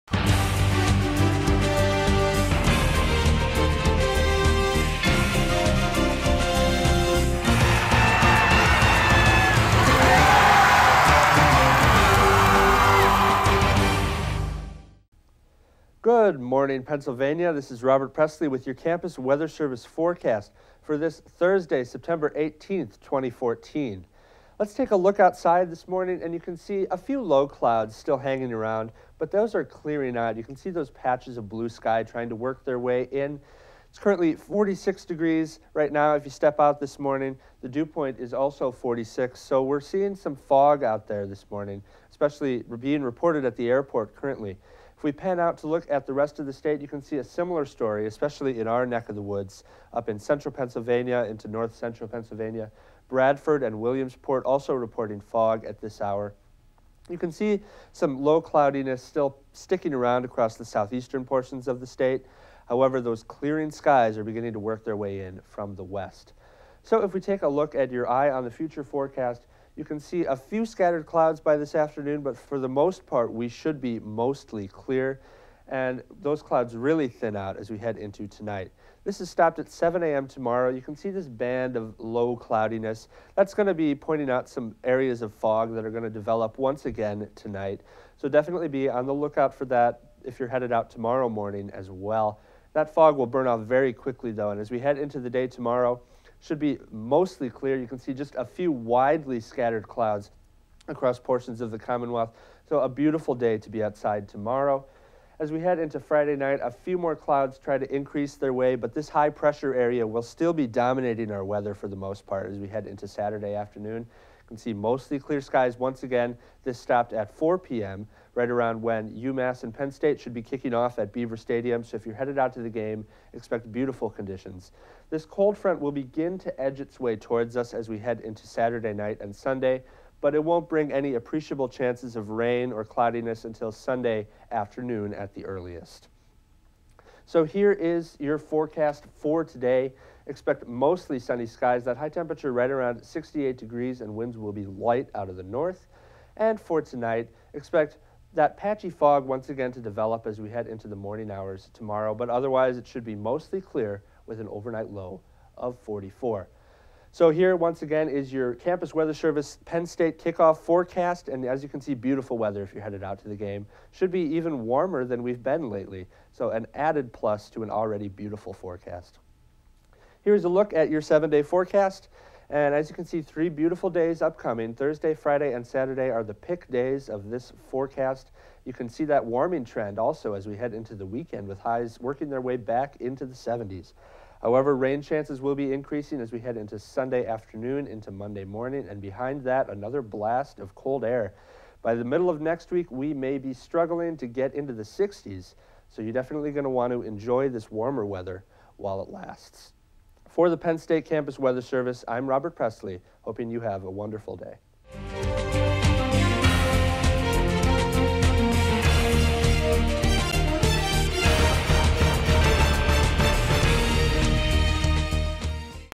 Robert Prestley's Thursday Morning Forecast
Robert Prestley gives the beautiful forecast, including what to expect for Saturday's Penn State - UMass game at Beaver Stadium